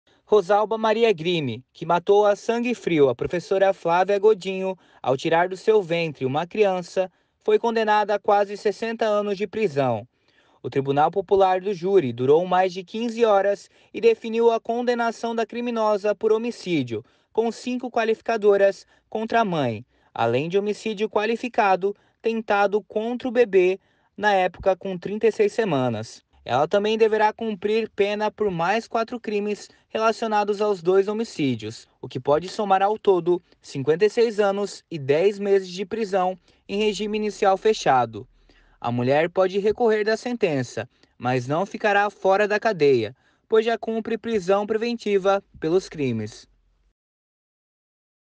Acusada de matar grávida de Canelinha pega 57 anos de prisão
Rozalba Maria Grime, que confessou ter matado uma grávida para roubar a bebê do ventre dela em Canelinha, na Grande Florianópolis, foi condenada a 56 anos e 10 meses de prisão na última quarta-feira (24).